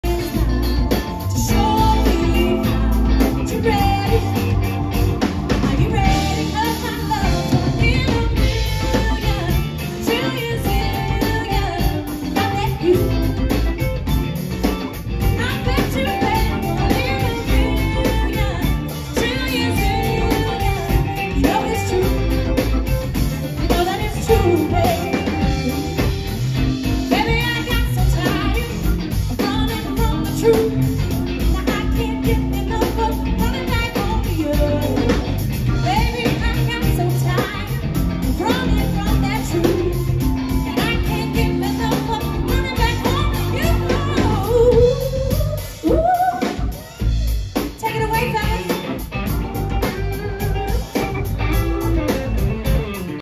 SBC 28-    Miss Jackie Wilson - Rolling Stone Lounge
January 18, 2024 on Sandy Beaches Cruise 28, Day 6